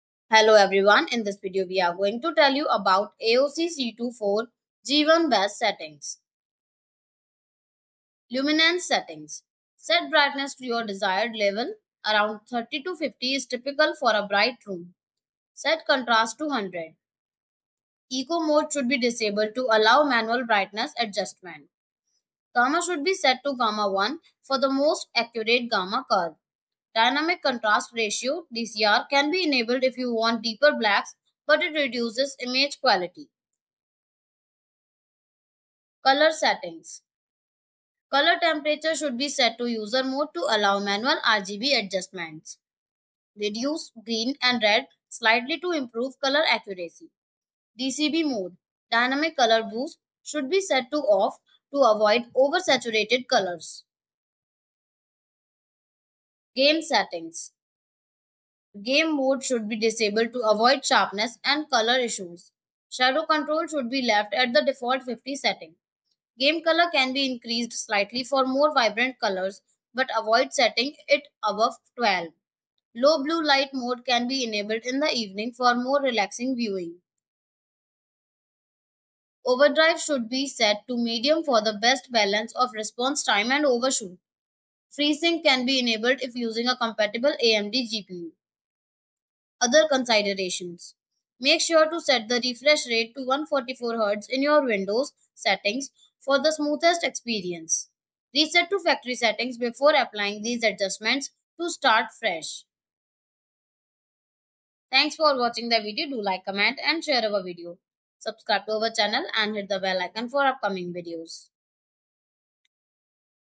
AOC C24G1 Best Settings
Welcome To Our Detailed Video On The Best Settings For The Aoc C24g1 Monitor. In This Video, We Will Provide Step-By-Step Instructions On How To Adjust Your Monitor For Superior Image Quality And Performance. From Fine-Tuning Brightness And Contrast To Achieving Accurate Color Representation, We Will Ensure That You Have All The Information Needed To Optimize Your Viewing Experience. Perfect For Gamers And Multimedia Enthusiasts Alike, This Video Guide Will Surely Help You To Unlock The Full Potential Of Your Aoc C24g1.

00:08 - Luminance Setting
00:36 - Color Settings
00:56 - Game Settings
01:29 - Other Considerations